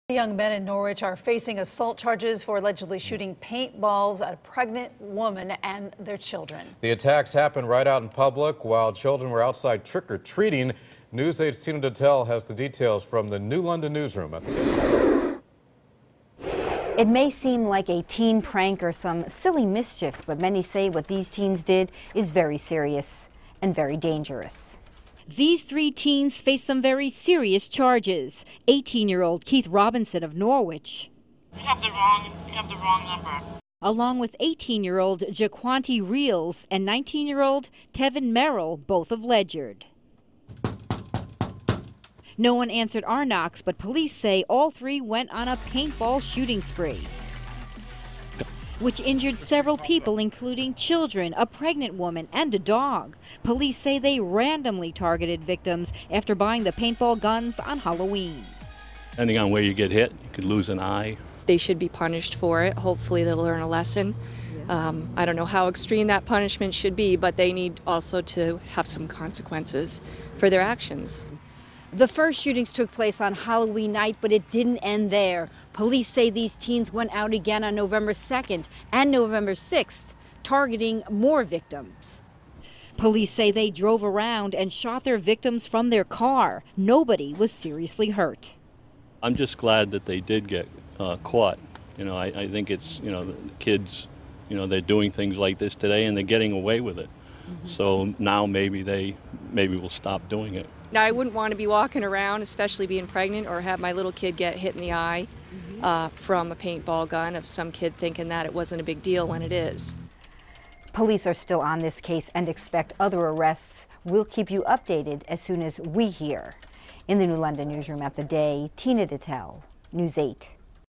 3 teens arrested for shooting paintballs
Three young men in Norwich are facing assault charges for allegedly shooting paintballs at pregnant women and their children.